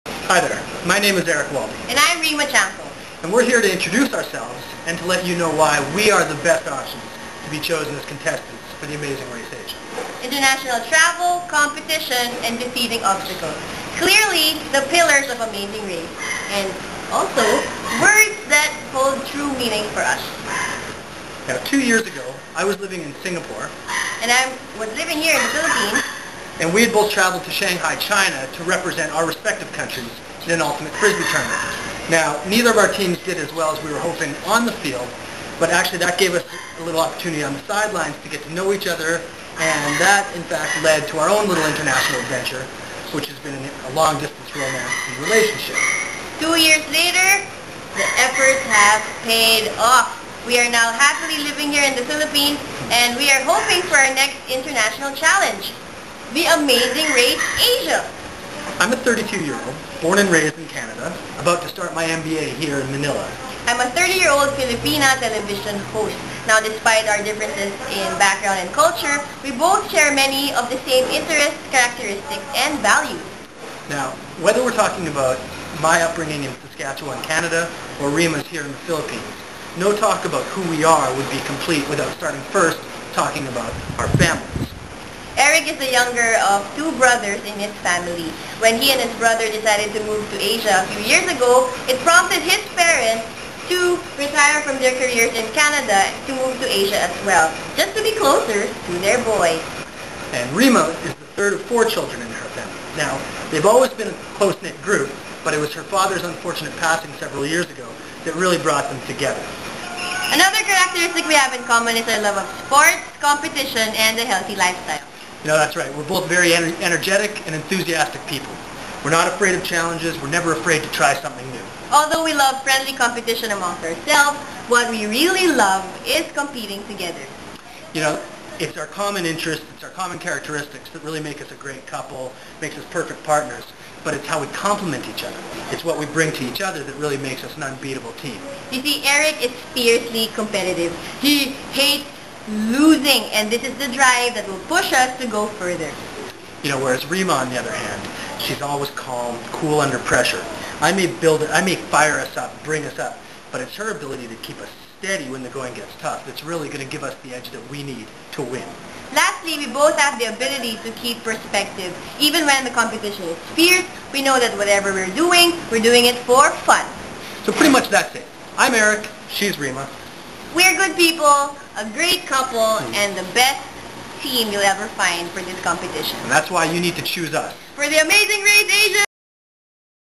Erik and Reema
our audition for the amazing race asia 2. keeping our fingers crossed but not holding our breath